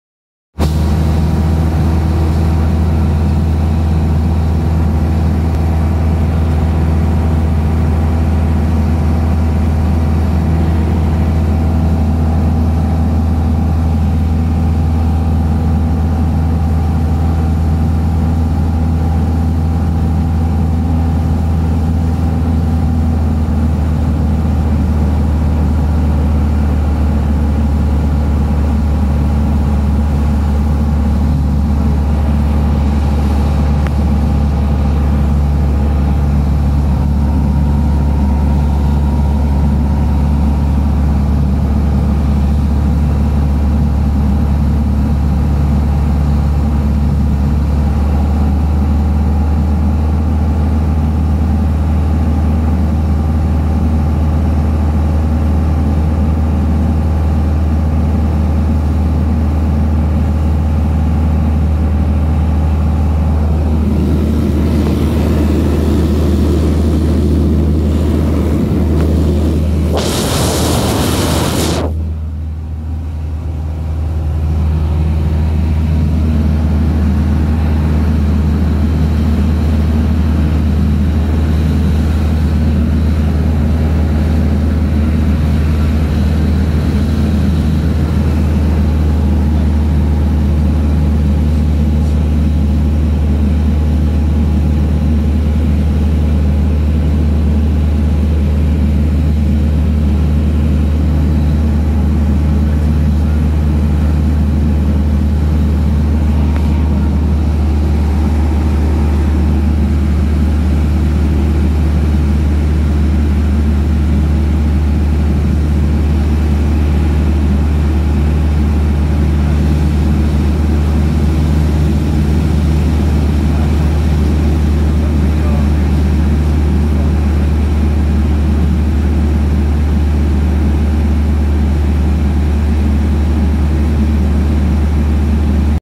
PA28 PPL VFR flight over LGW London Gatwick Airport at 1200 feet
Massive thanks to the Gatwick Director for the special permission to fly this heading and height. Once in a lifetime?